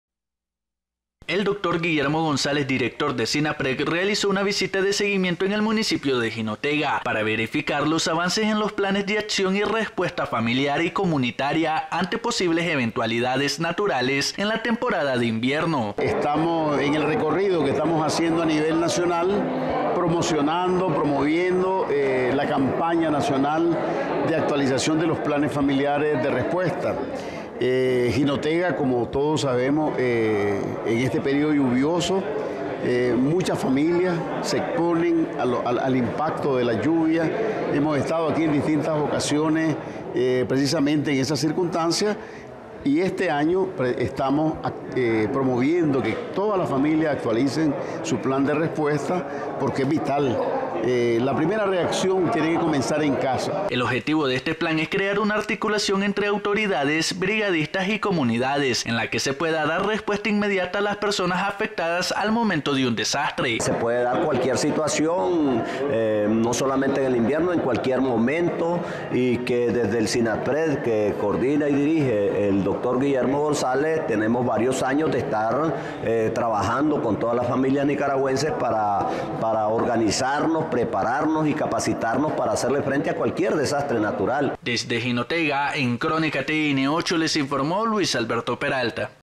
SINAPRED fortalece planes de acción de respuesta familiar en Jinotega - Nicaragua
En el auditorio Julio López Montenegro de Jinotega, se congregaron los comités (Policía Nacional, Bomberos, Alcaldía, Instituciones, Cruz Roja, Ejército y secretarios de barrio), que unidos conforman la delegación de COMUPRED de Jinotega; esto para formar parte de una sesión especial de acompañamiento y seguimiento por parte del Sistema Nacional de Prevención de Desastres SINAPRED.

El Dr. Guillermo González, Ministro Director del SINAPRED, realizó una visita de seguimiento en el municipio de Jinotega; para verificar los avances en los planes de acción y respuesta familiar y comunitaria ante posibles eventualidades naturales en la temporada de invierno.

Leónidas Centeno, Alcalde de Jinotega, manifestó: «Estamos aquí todos reunidos para seguir avanzando más en el proceso de fortalecimiento de los planes que; juntos tenemos que organizados; para enfrentar cualquier desastre que la naturaleza nos presente en más de 200 puntos críticos en todo el departamento de Jinotega».

También expresó, «Este plan de respuesta familiar no solo es Jinotega como municipio; es también todo el departamento, y Dios mediante, sabremos enfrentar cualquier eventualidad que se nos presente en el camino».

El objetivo de este plan es crear una articulación entre autoridades, Brigadistas y comunidades; para que se pueda dar respuesta inmediata a las personas afectadas al momento de un desastre.

Planes de acción en Jinotega
Por su parte, el doctor Guillermo González de SINAPRED expresó: «Estamos hoy aquí en Jinotega; dando seguimiento y acompañamiento de parte de nuestro buen gobierno; para ver los avances que llevamos hasta el día de hoy en la ejecución de los planes de acción; respuesta familiar y comunitaria ante posibles eventualidades que se produzcan antes de la entrada del invierno con lluvias fuertes; o bien con tormentas tropicales».

«Dios mediante la naturaleza no nos sorprenda, pero si lo hace que estemos listos para acudir a la acción y dar respuesta», finalizó González.

Las autoridades del COMUPRED asumieron su compromiso en seguir trabajando; a fin de fortalecer a las familias y comunidades ante posibles eventualidades.